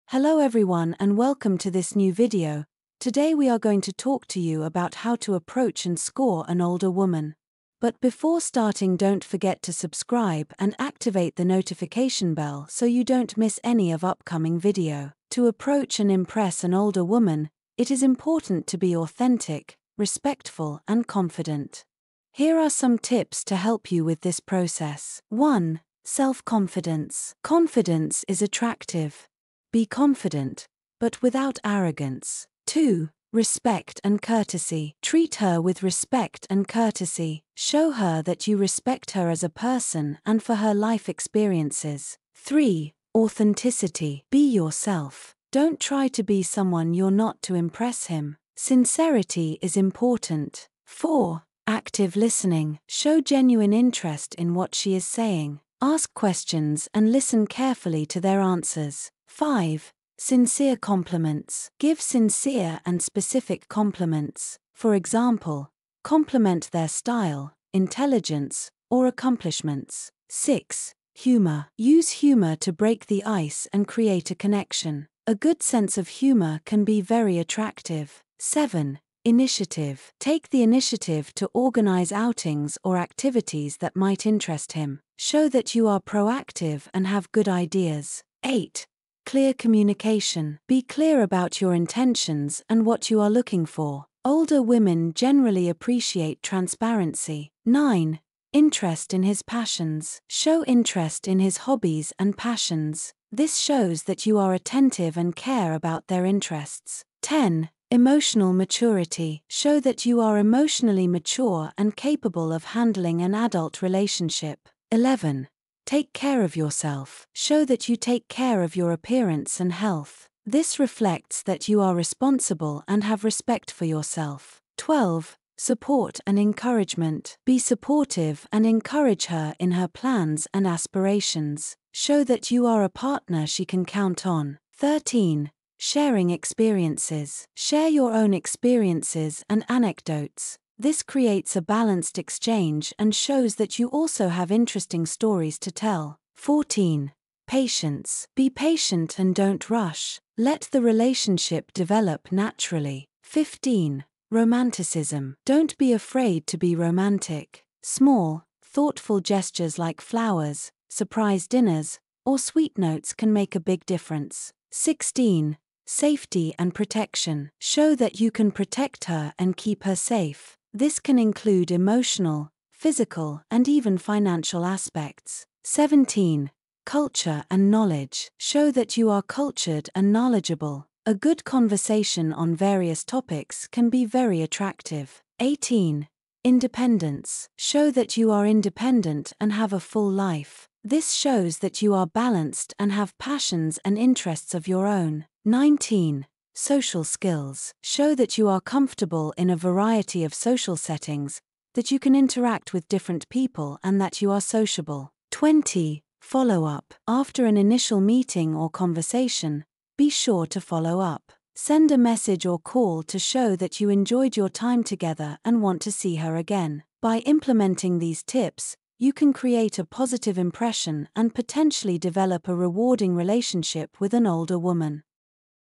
How to approach a woman older than you and succeed date with her
In this video, we are talk about How to approach a woman older than you and succeed date with her